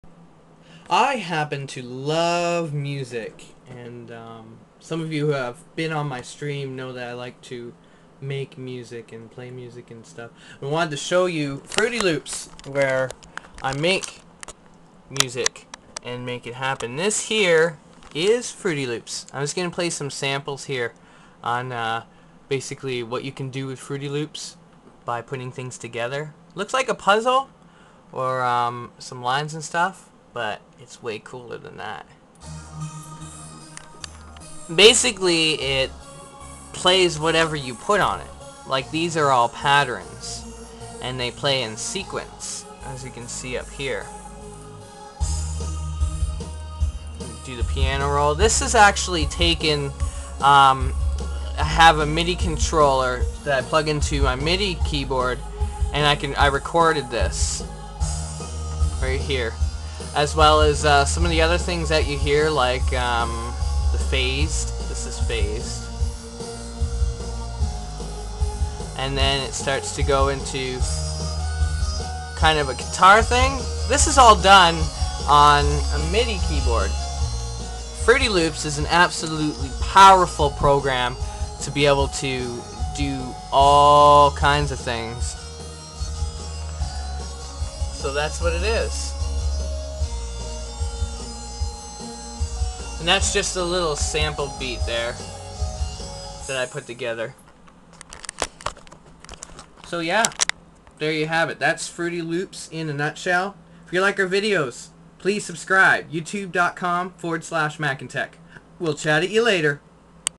FL Studio Stuff - Making Your Own Music
Fruity Loops is a powerful program that allows you to make your own music, all on the computer!